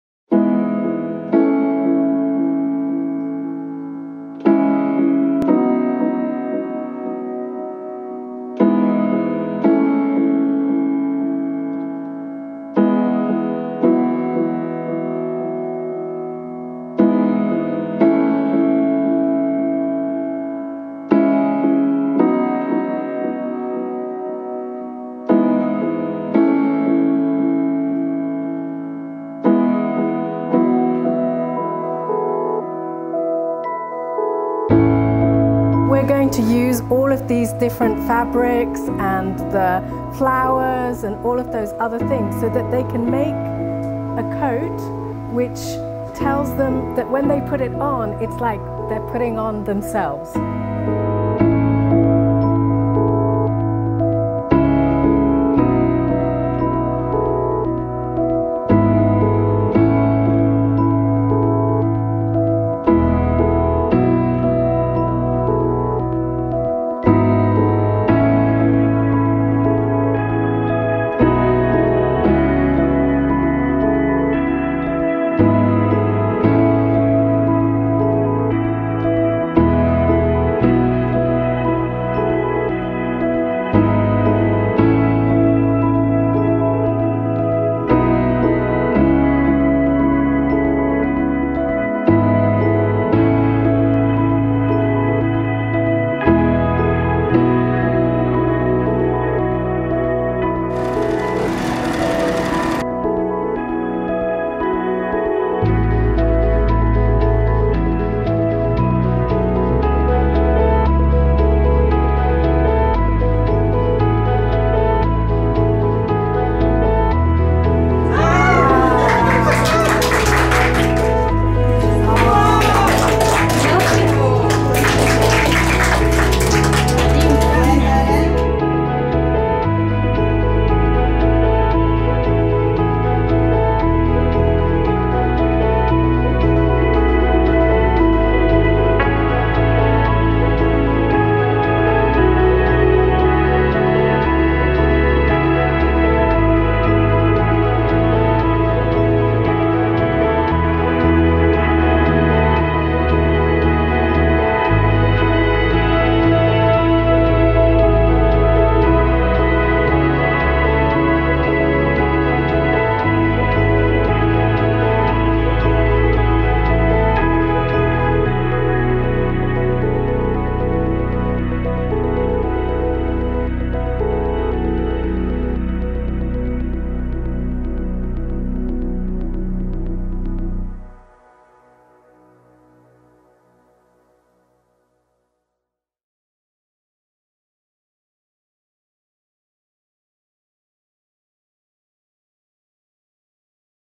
Dress For Our Time - LOVECOATS Project with the TIGER girls
Working alongside the refugees, Professor Helen Storey's Dress For Our Time projects continue to evolve at Za'atari Refugee Camp in Jordan.

The LOVECOATS project came out of a conversation with the Tiger Girls last year. a project designed with them and inspired by their hopes and wishes for the future. They told Helen of their love of fashion , their wish for making skills, and of their wish to be able to gift to others as well as of their fear of the cold winters in the camp. All these things were addressed through this collaborative project and each coat they made was as imaginative and individual as each of the Tiger girls themselves.